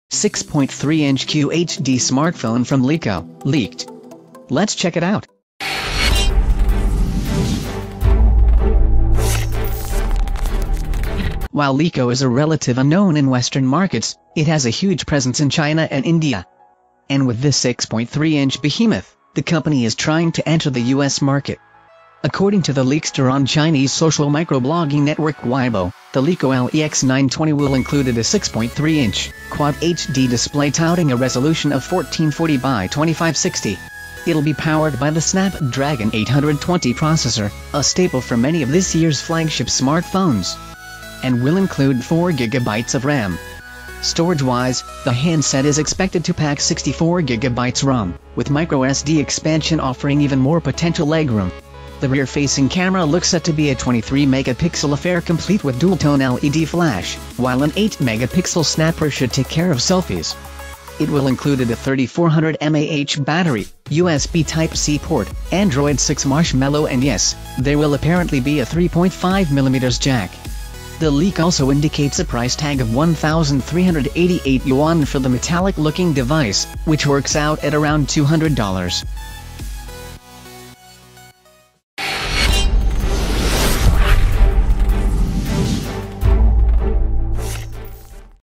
LeEco LeX920 LEAKED - 6.3" QHD Display, 4GB RAM, Snapdragon 820 and more
LeEco LeX920 has pretty great specifications for a $200 phone. They have a strong presence in China and India and with this phone, they're trying to get into the western market.